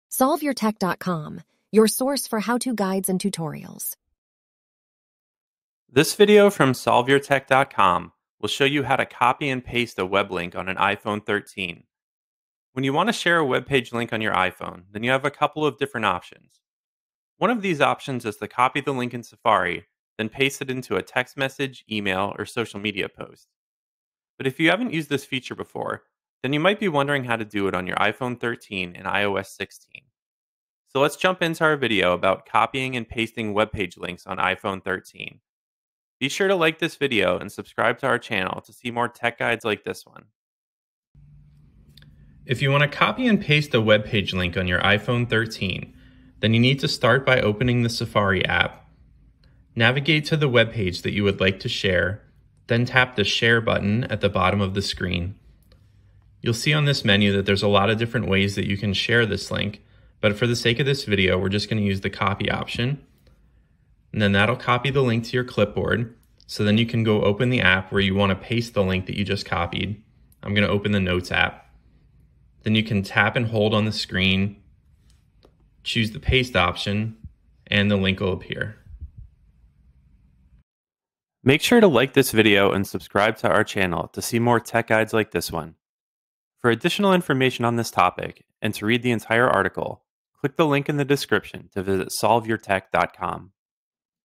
iPhone 13 - How to Copy and Paste a Web Link
Are you struggling with copying and pasting a web page link on your iPhone 13's Safari browser? Look no further! In this comprehensive guide, we'll cover everything you need to know to copy and paste a web page link with ease.

Firstly, let's go over the basics. To copy a web page link on Safari, you'll need to go to the page you want to share, then tap the Share icon at the bottom of the screen. From there, you can select "Copy" to save it to your clipboard. To paste the link, press and hold on a text field until the "Paste" option appears, then select it to insert the link.

But what about copying and pasting multiple links or sharing links with others? There are a few additional tips and tricks you can use to streamline the process. For instance, you can copy multiple links at once by opening each link in a new tab and then tapping and holding on one of the tabs to select "Close Tabs" and "Open Tabs in Other Devices." This will open the tabs on another device, such as your iPad or MacBook, where you can then copy all of the links at once.

If you frequently share links with others, you may want to consider using a third-party app such as Pocket or Instapaper. These apps allow you to save and organize links for later viewing, as well as share them with others via email, text message, or social media. To share a link from one of these apps, simply select the "Share" option and choose your preferred method of sharing.

It's also worth noting that you can copy and paste links from other apps into Safari. For example, if you receive a link via text message or email, you can simply press and hold on the link and select "Copy Link" to save it to your clipboard. Then, switch over to Safari and paste the link as described earlier.

Overall, copying and pasting web page links on Safari is a simple process once you know the basics. By using the tips and tricks outlined in this guide, you can streamline the process and save yourself time and hassle in the long run.

So there you have it – everything you need to know about copying and pasting web page links on Safari for iPhone 13. With these tips and tricks, you'll be a pro in no time!

00:00 Introduction
00:41 Overview of How to Copy and Paste Links on an iPhone 13
01:20 Outro